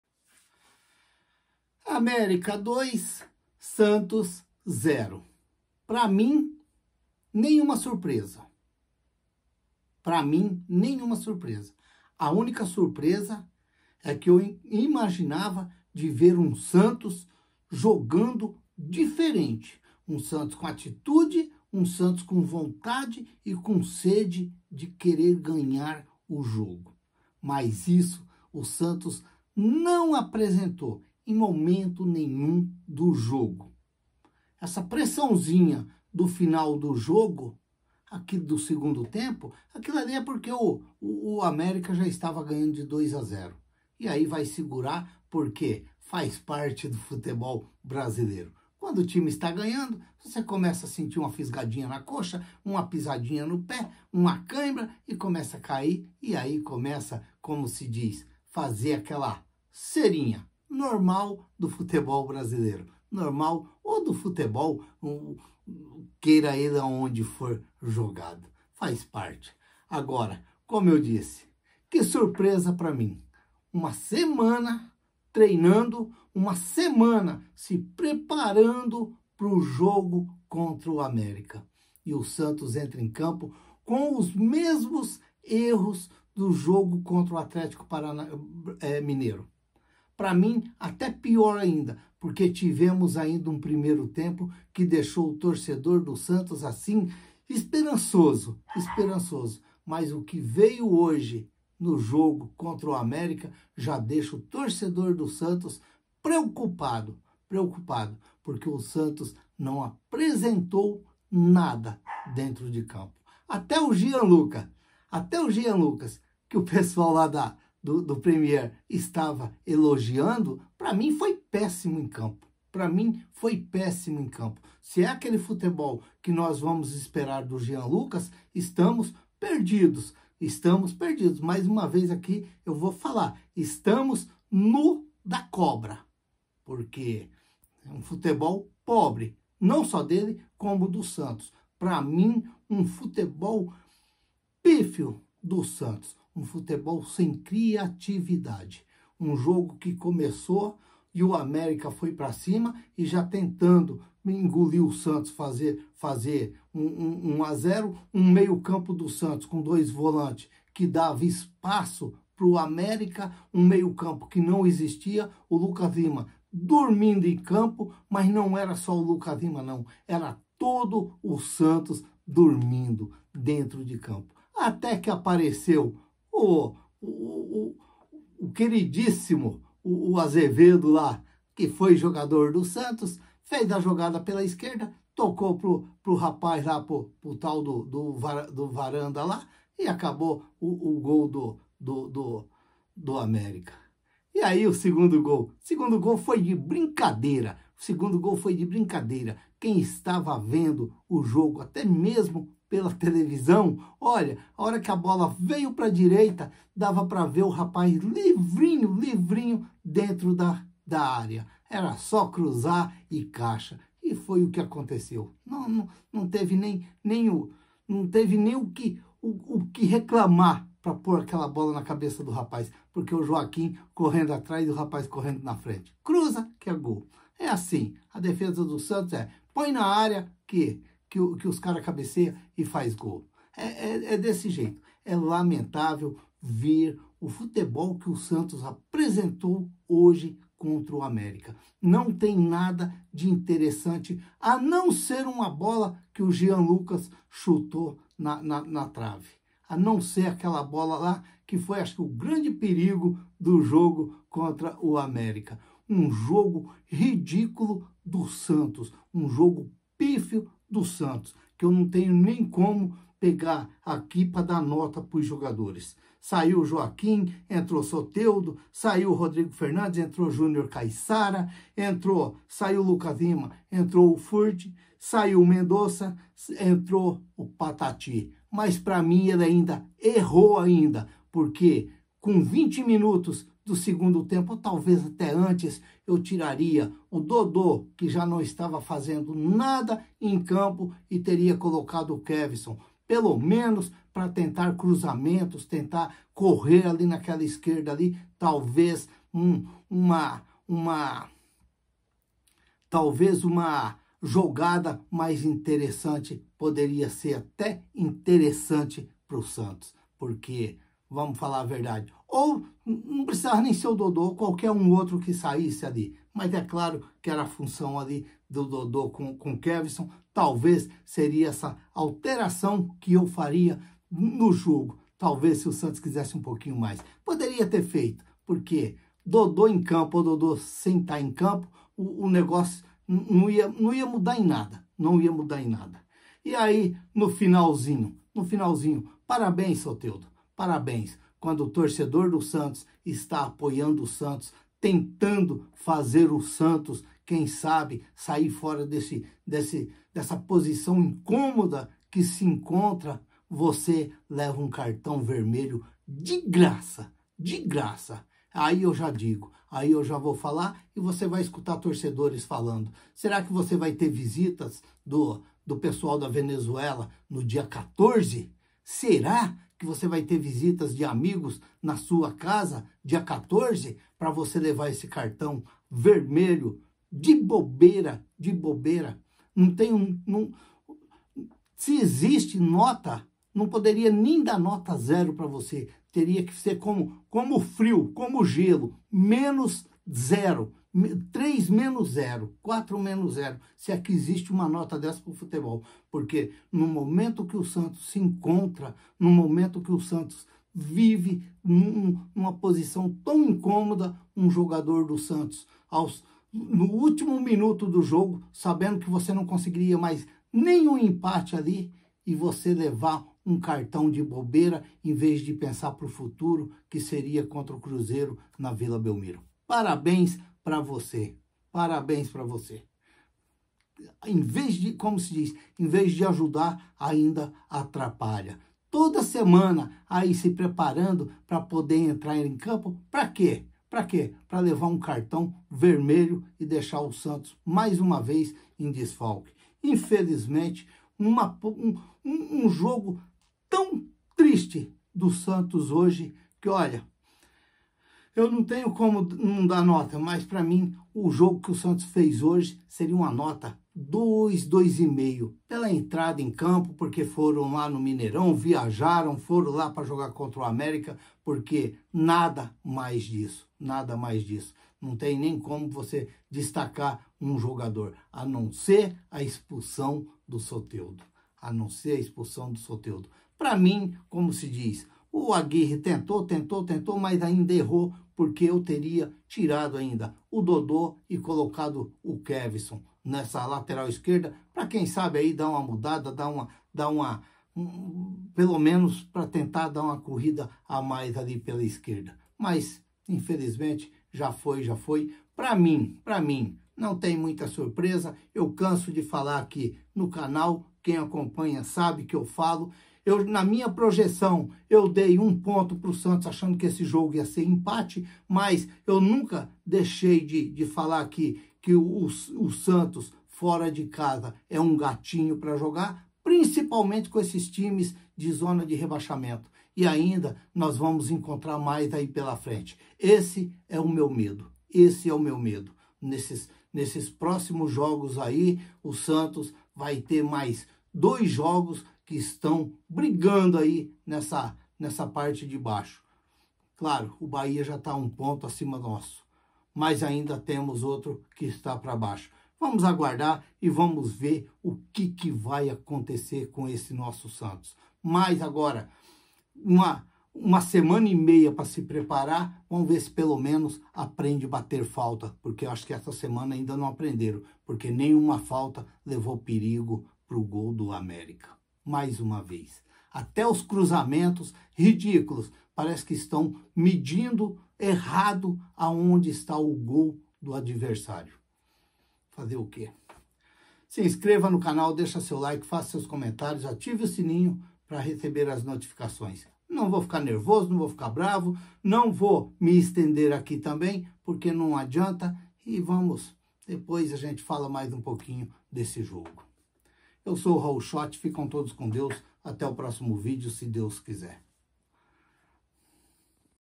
Pós jogo América 2 x 0 Santos futebol ridículo do peixe no independência
Na Tarde deste domingo no Independência o Santos foi derrotado pelo América Mineiro por dois a zero no jogo onde o Santos fez uma péssima partida e não conseguiu enfrentar o coelho de igual pra igual muitos problemas no Santos e Diego aguerri vai ter que trabalhar muito durante essa semana pra quem sabe deixar esse Santos preparado para enfrentar o próximo jogo que será o Cruzeiro.se inscreva no canal peixenautas.futebol peixenautas santos santosdomundo santosfc santosfutebolclube vilabelmiro campeonatobrasileiro soteldo américa soteldo diegoaguirre ￼